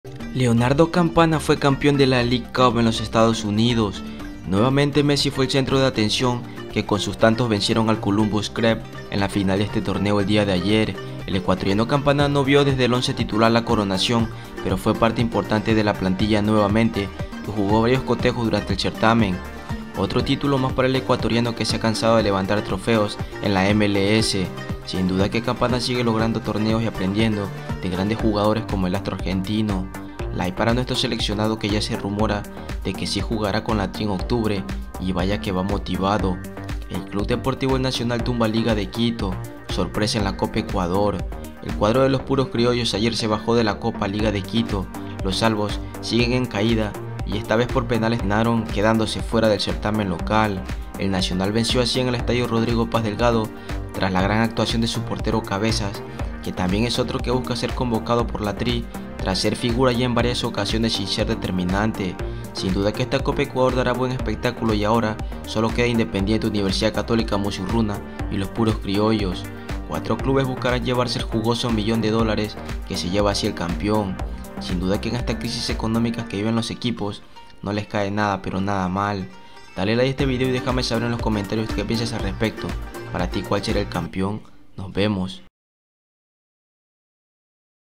QUE NIVEL! EL NACIONAL baja a LDU y VA POR LA COPA ECUADOR |CAMPANA CAMPEON EN MLS
*SI DESEAS COLABORAR CON EL CANAL , PUEDES HACERLO MEDIANTE LA OPCIÓN "GRACIAS" DISPONIBLE EN LA PARTE BAJA DE LOS VIDEOS*